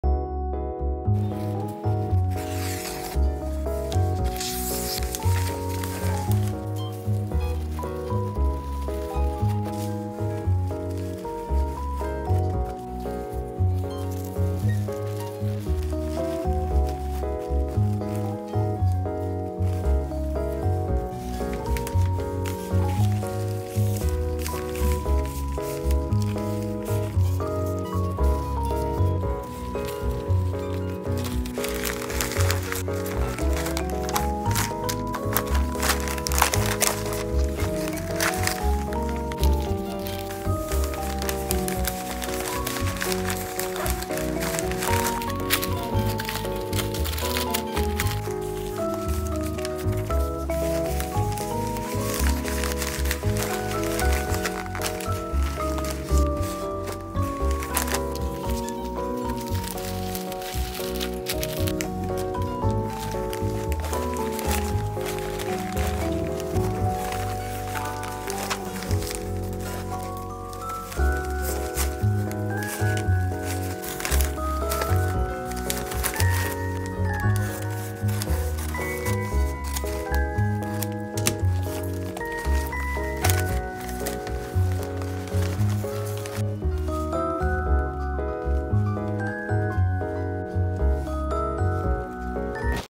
꽃다발 포장하는법 꽃선생님
꽃집에서는 어떻게 꽃다발을 제작하고 만들까요? 꽃다발포장하는법
핸드타이드를 마무리한뒤에 포장하는 모습을 담아보았습니다.
겨울이지만 다양하고 따뜻한 색감의 꽃들 덕분에 즐겁게 포장했어요~
하루가 길었네요. 오늘도 모두모두 수고많으셨습니다.